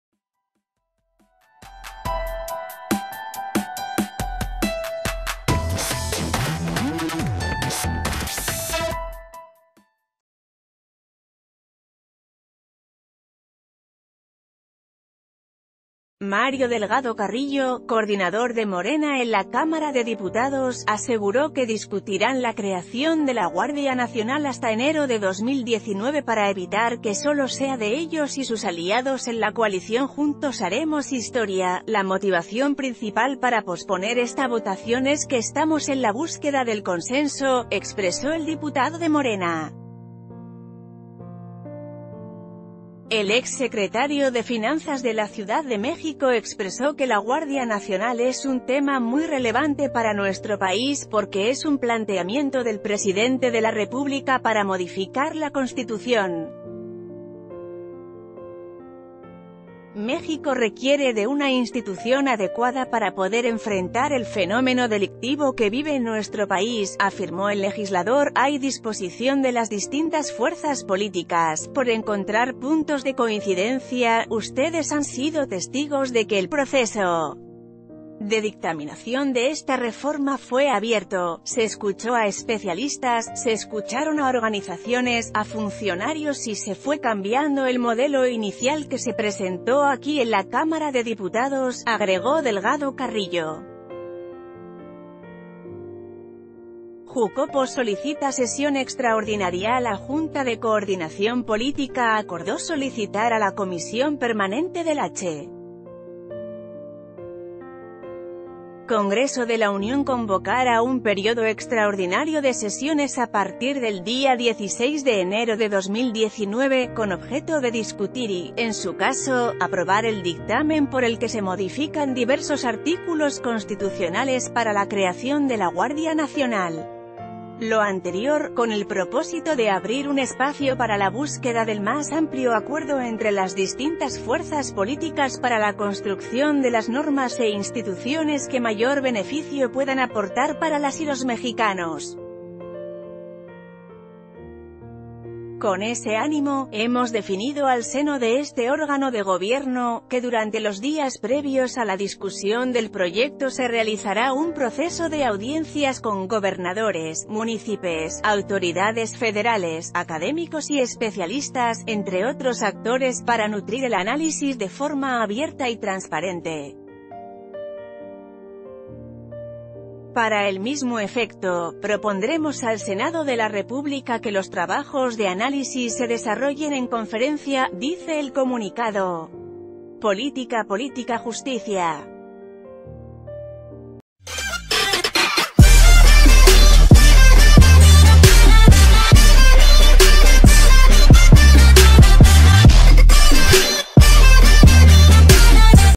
No queremos que la Guardia Nacional sea solo de Morena: Mario Delgado
Mario Delgado Carrillo, coordinador de Morena en la Cámara de Diputados, aseguró que discutirán la creación de la Guardia Nacional hasta enero de 2019 para evitar que sólo sea de ellos y sus aliados en la coalición Juntos Haremos Historia.“La motivación principal para posponer esta votación es que e...